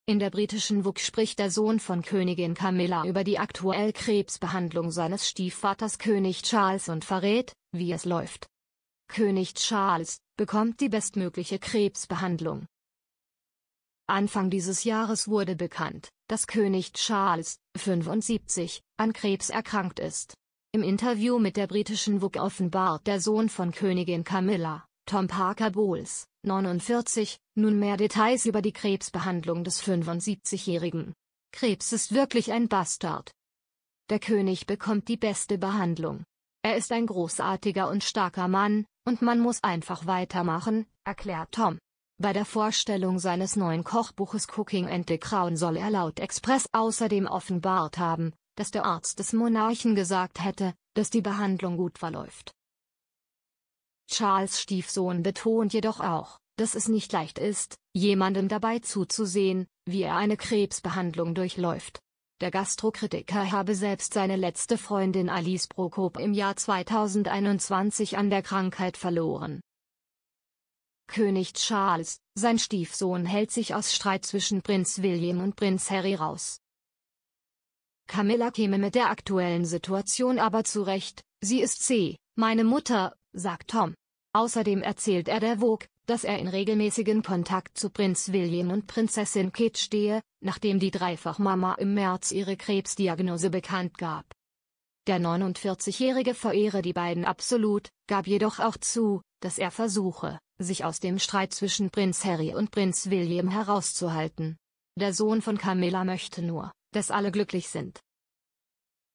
König Charles Stiefsohn enthüllt Details zu Krebsbehandlung
In der britischen Vogue spricht der Sohn von Königin Camilla über die aktuelle Krebsbehandlung seines Stiefvaters König Charles und verrät, wie es läuft.